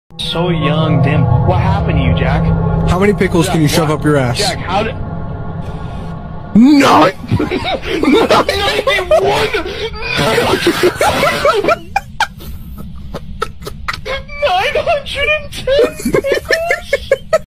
how many pickles can you shove up your 🏳️‍⚧️? || GL2
❥ not canon not adding ghosts to the lore btw

I will be trying to make some skits like actual skits that will show more of the characters' psychological problems and also I meed to rewrite quite a lot of lore cus I wanna bring these characters back (for views /j)

❥ Characters used ::

Adelmar Void (black & white hair) :: they/he

Amaziyah Obonadgo (poc girl) :: she/her

Cade Garret (g*nger) :: he/him